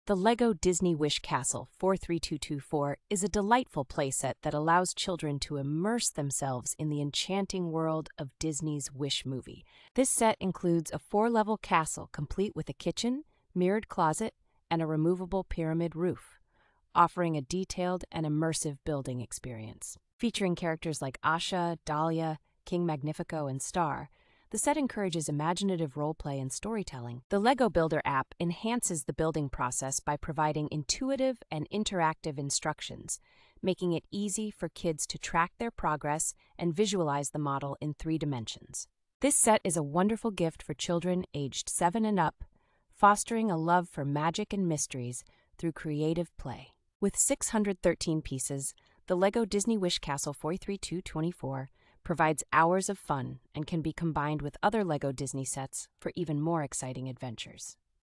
REVIEW (2024): LEGO Disney Wish Castle 43224. ESSENTIAL details.
The LEGO Disney Wish Castle 43224 is a delightful playset that allows children to immerse themselves in the enchanting world of Disney's Wish movie.

This set includes a four-level castle complete with a kitchen, mirrored closet, and a removable pyramid roof, offering a detailed and immersive building experience.

Featuring characters like Asha, Dahlia, King Magnifico, and Star, the set encourages imaginative role-play and storytelling.

The LEGO Builder app enhances the building process by providing intuitive and interactive instructions, making it easy for kids to track their progress and visualize the model in three dimensions.

This set is a wonderful gift for children aged seven and up, fostering a love for magic and mysteries through creative play.

With six hundred thirteen pieces, the LEGO Disney Wish Castle 43224 provides hours of fun and can be combined with other LEGO Disney sets for even more exciting adventures.